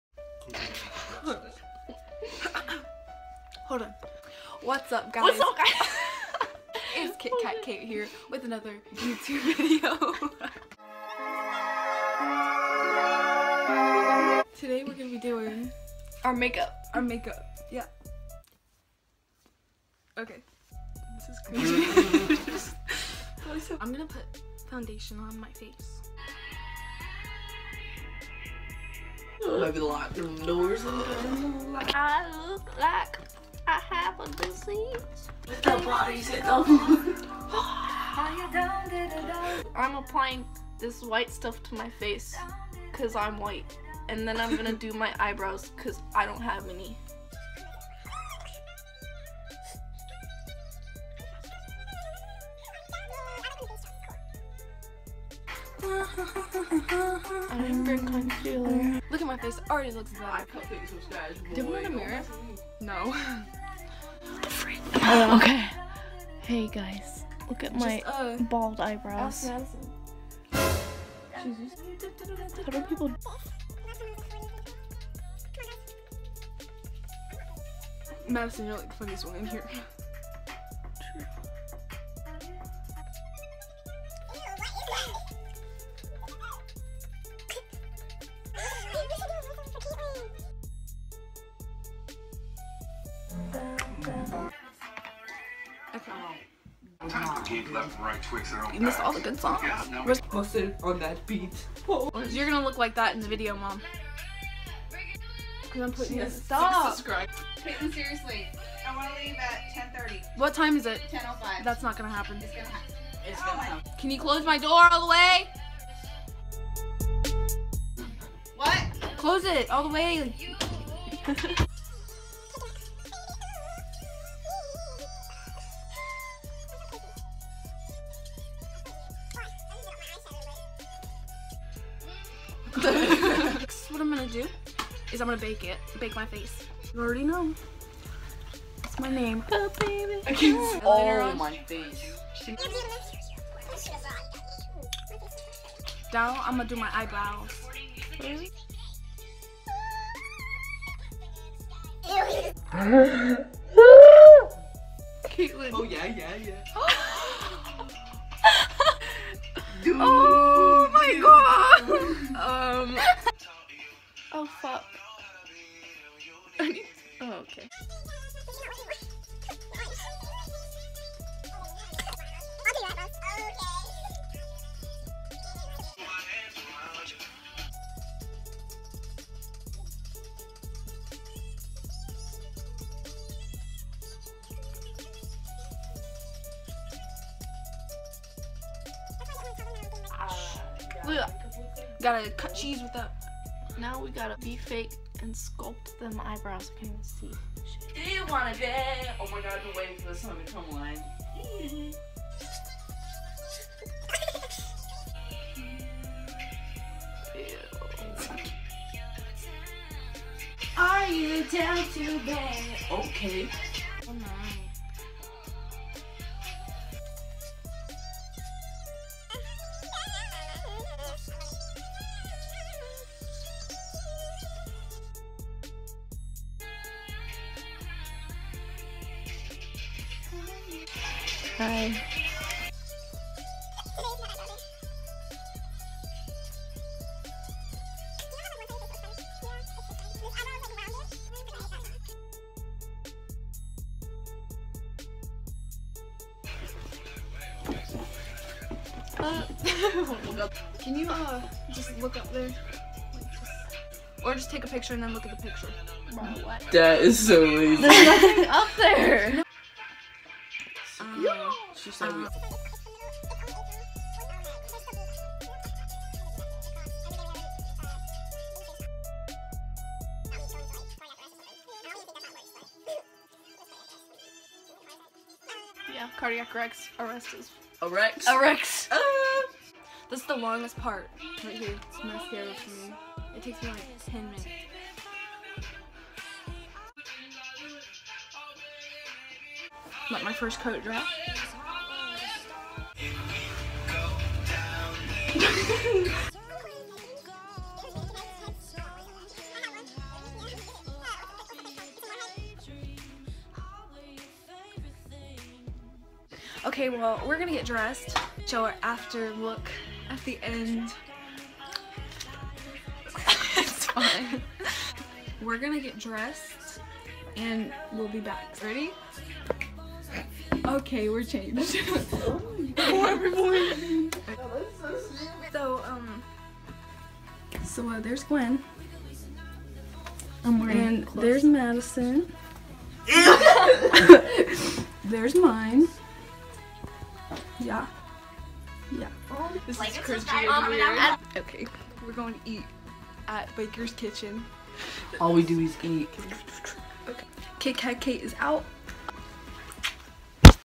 Makeup Tutorial!! || Getting Ready With Friends Routine!!
Just your basic girls getting ready...how it really goes 😂